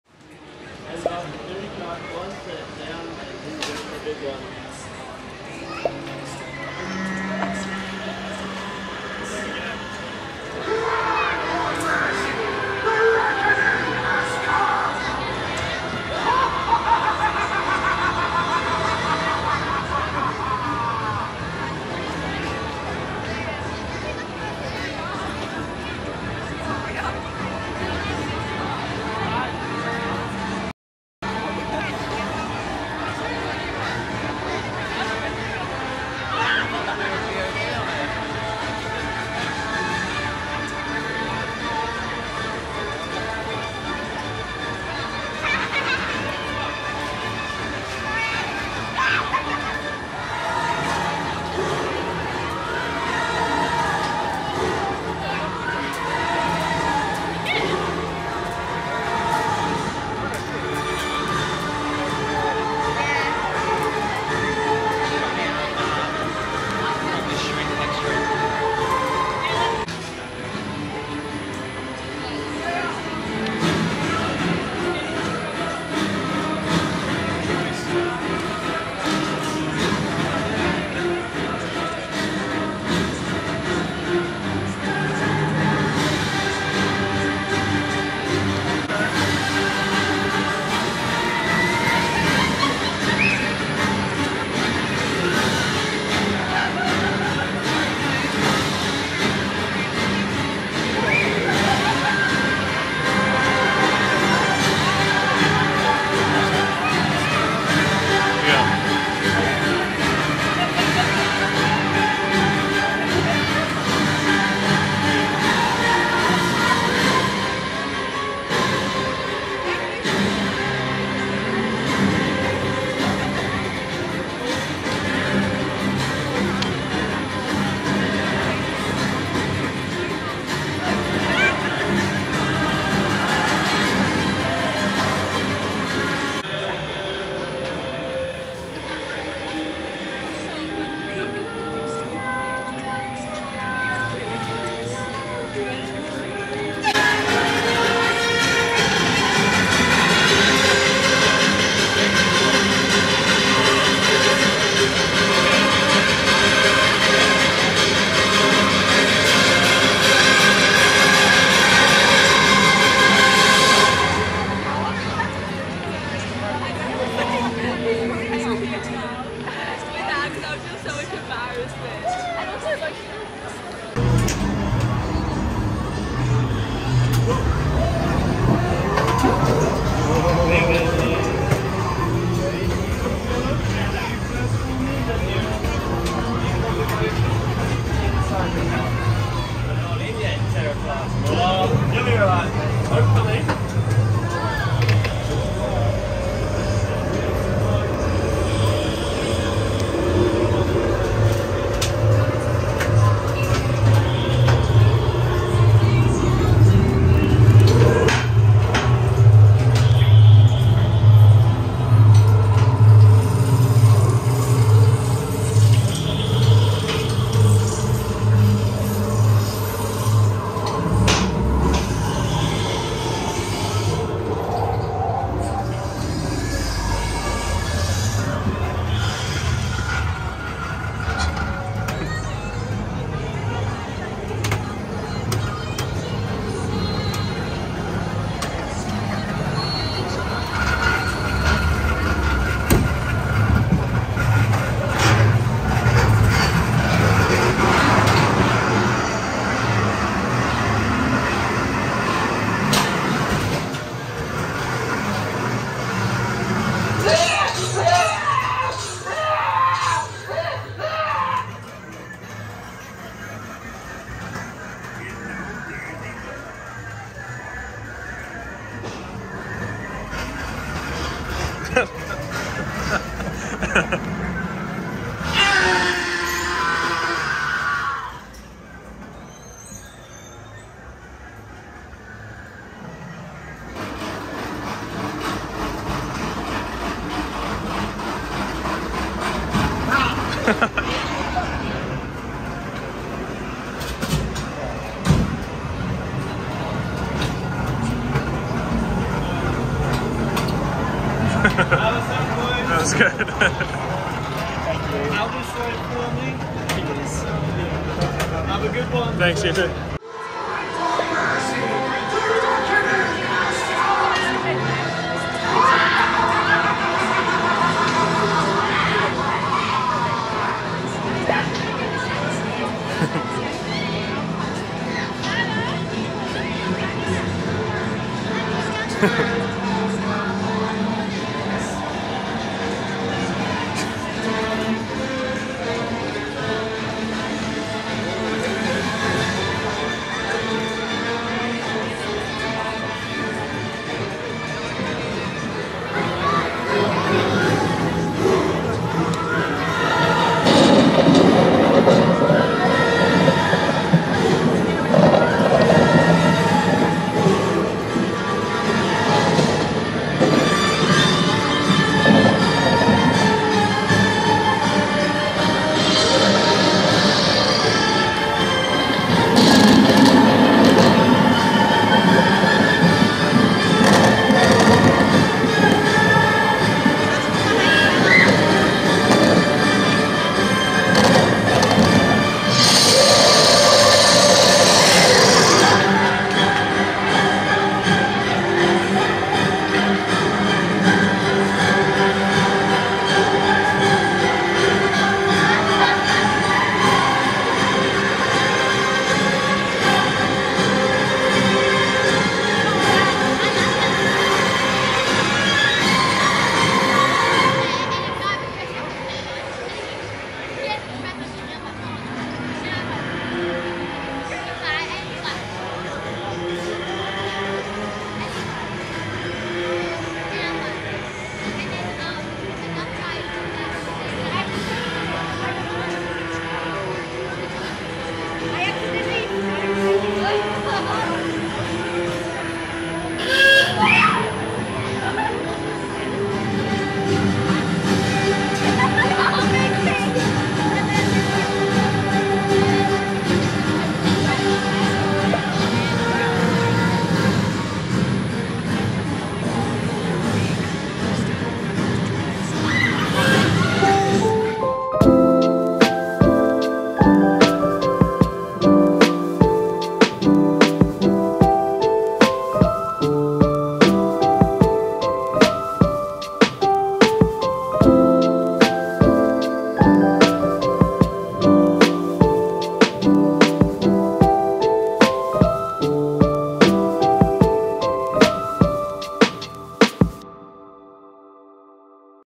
Spook Ghost Train POV - Brisbane Royal Show - EKKA 2024 - Off Ride & On Ride Clips - Day & Night 4K
We visited the EKKA 2024 and checked out the Spook Ghost Train again! We didn't get to ride it this year, but I recorded lots of off-ride footage

0:00: Off-Ride Day Time
2:53: On Ride POV
5:11: Off-Ride Night Time

If you love travel, adventure, food, and theme parks.

Off-ride footage filmed on Sony ZV-E10 (S-log 2)
POV filmed on Iphone 13 Pro.